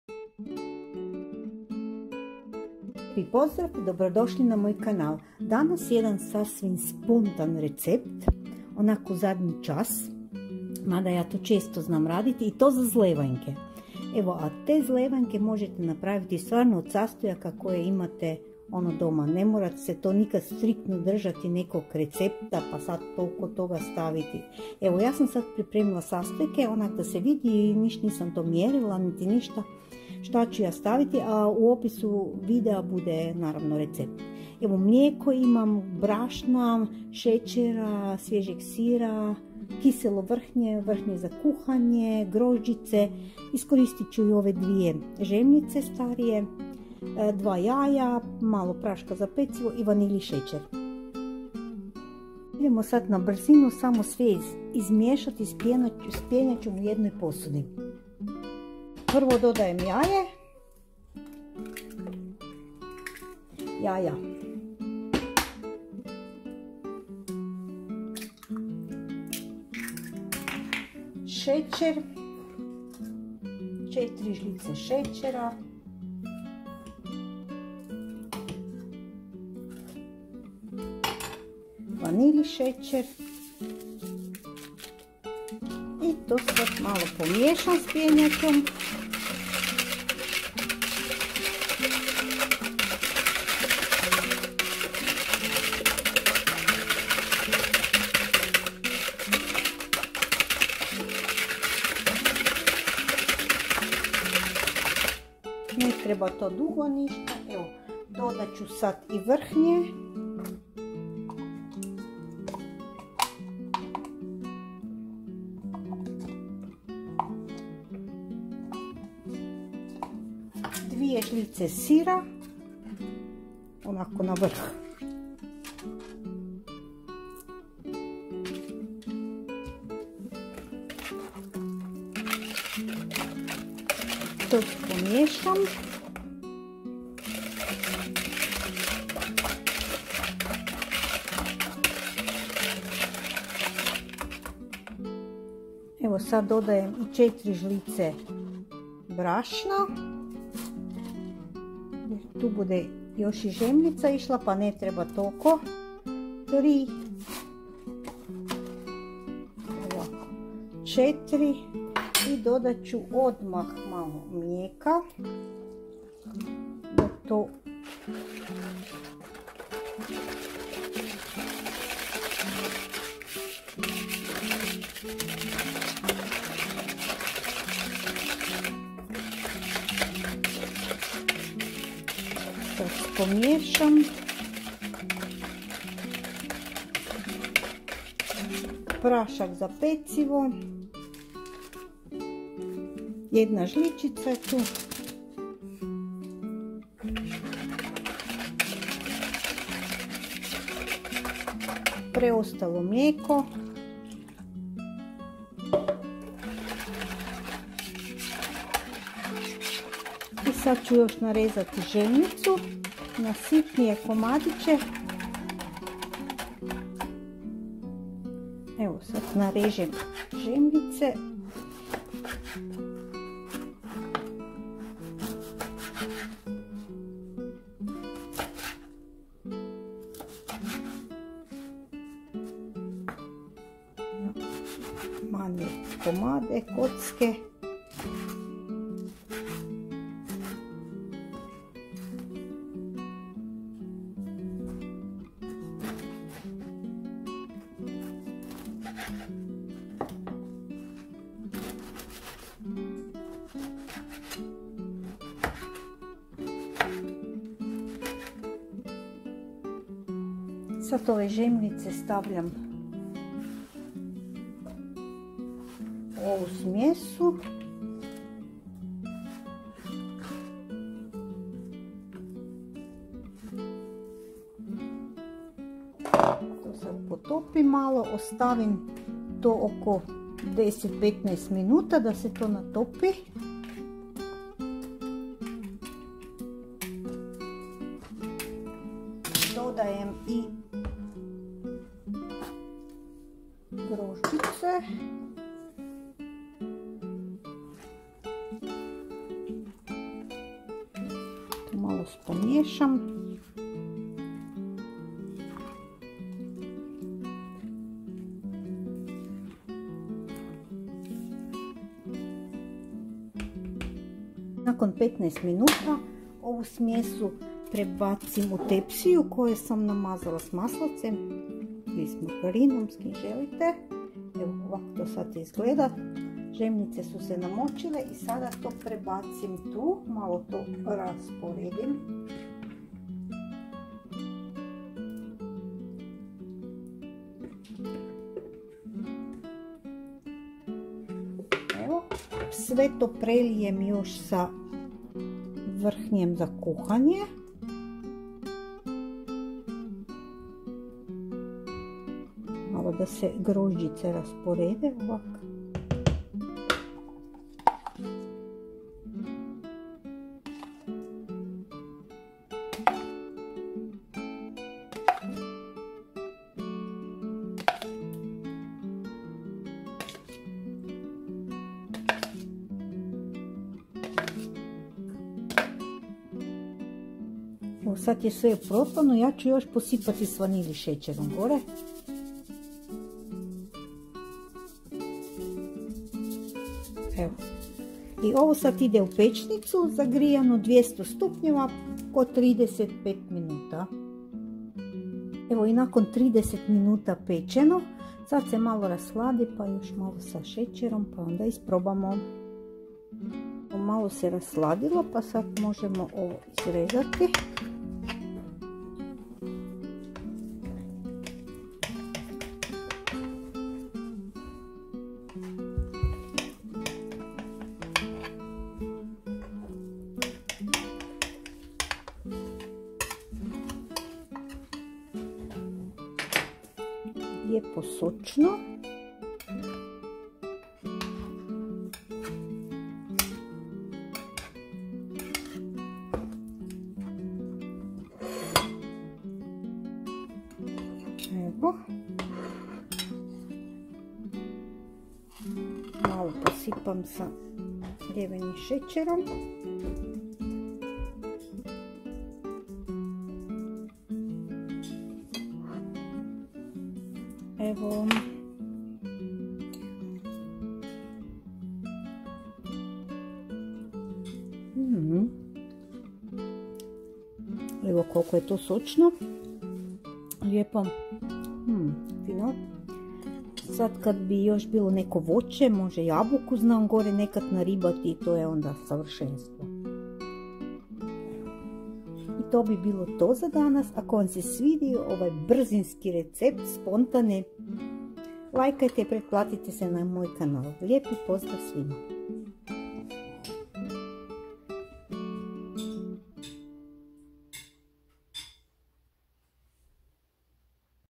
Jednostavan desert/Einfaches Dessert/Simple dessert recipe
Ako želite nešto brzinski slatko ali i jednostavno, evo jednog takvog spontanog recepta.😃Lp 😘

2 jaja/2 Eier/2 eggs
4 žlice šećera/4 EL Zucker/4 tbsp sugar
2 vanilin-šećera/2 Vanillezucker/2 vanilla sugar
Prstohvat soli/1 Prise Salz/a pinch of salt
200 g kiselog vrhnja/saure Sahne/sour cream
2 žlice sira/2 EL Quark/2 tbsp quark
500 ml mlijeka/Milch/milk
1 žličica praška za pecivo/1 TL Backpulver/1 tsp baking powder
4 žlice brašna/4 EL Mehl/4 tbsp flour
3 žlice grožđica/3 EL Rosinen/3 tbsp dried raisins
2 žemljice(stare)/2 alte Brötchen oder Brot/2 old rolls or bread
250 ml vrhnja za kuhanje/Kochsahne/Cooking cream
Malo maslaca/etwas Butter/some butter
Mljeveni šećer/Puderzucker/powdered sugar
Vrijeme pečenja/Backzeit/Baking time:
30 min.-200*C